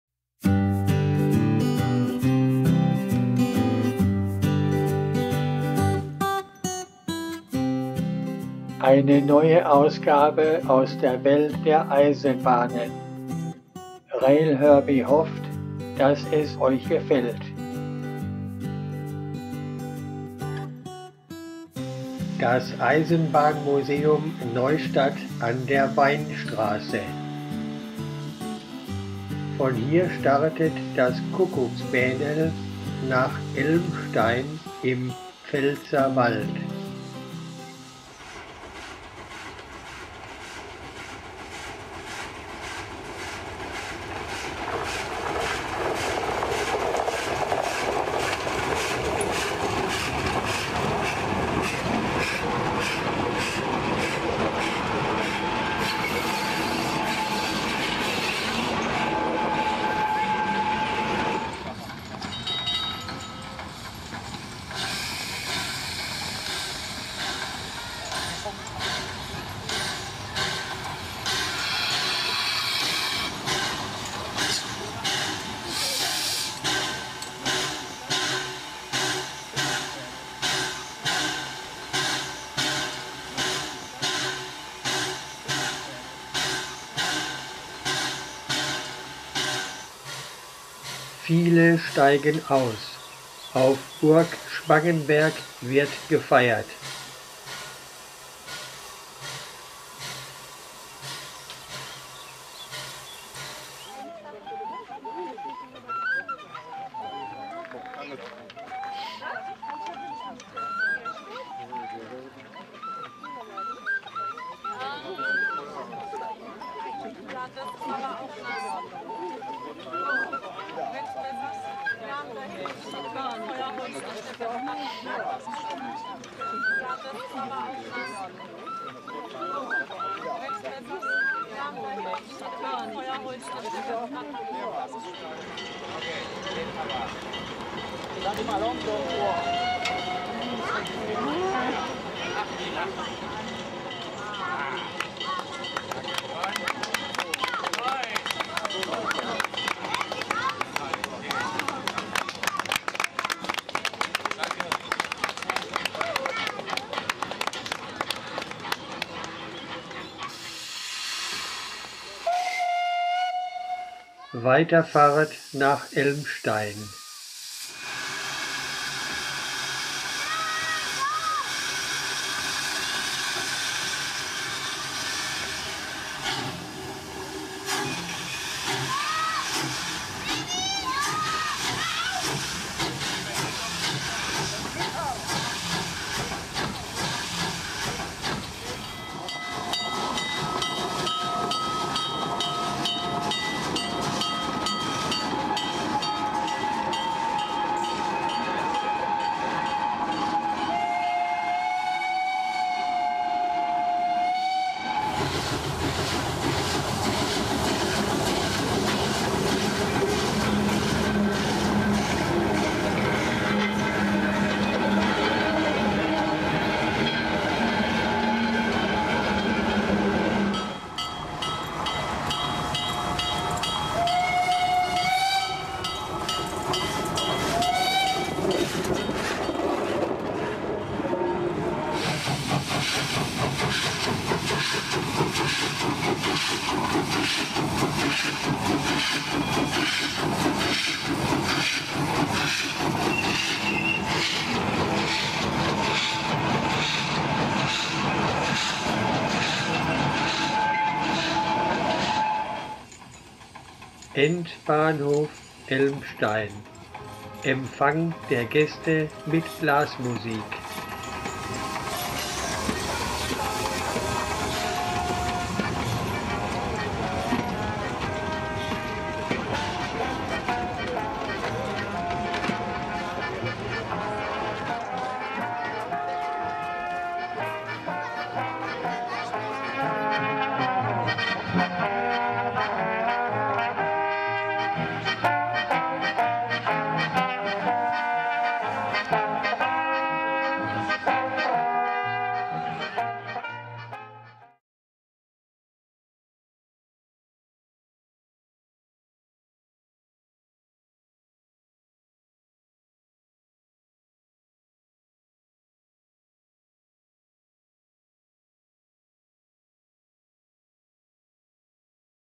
#081: Deutschland - Kuckucksbähnel (Germany - Kuckucksbähnel)
Das Kuckucksbähnel ist eine normalspurige Nebenbahn von Lambrecht bei Neustadt an der Weinstraße nach Elmstein im Pfälzer Wald. Die Strecke wurde 1909 eröffnet, ist 13 km lang und seit 1984 Museumsbahn.
Euer Railherbie alias Herbert Krause
The Kuckucksbähnel is a normal gauge railway from Lambrecht close to Neustadt at the Weinstraße to Elmstein in the Pfälzer Forest. The route had been opened in 1909, is 13 km long and since 1984 museum railway.
Yours Railherbie alias Herbert Krause